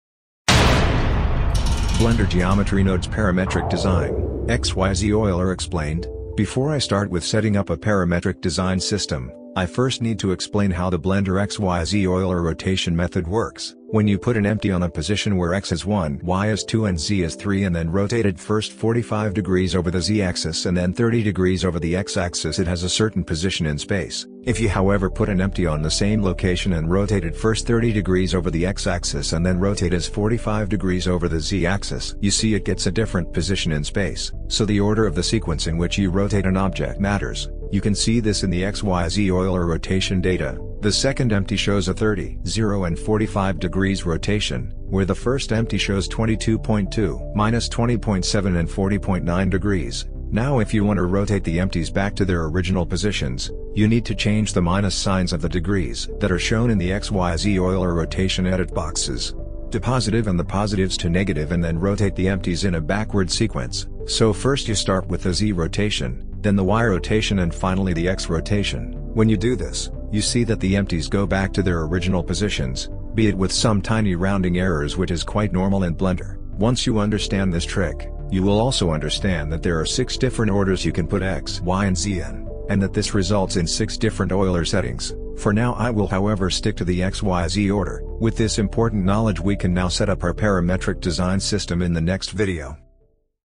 Blender Geometry Nodes Parametric Design - XYZ Euler Explained
Before I start with setting up a Parametric Design system, I first need to explain how the Blender XYZ Euler Rotation method works. Once you understand this trick, you will also understand that there are 6 different orders you can put X, Y and Z in, and that this results in 6 different Euler settings. With this important knowledge we can now set up our Parametric Design system in the next video.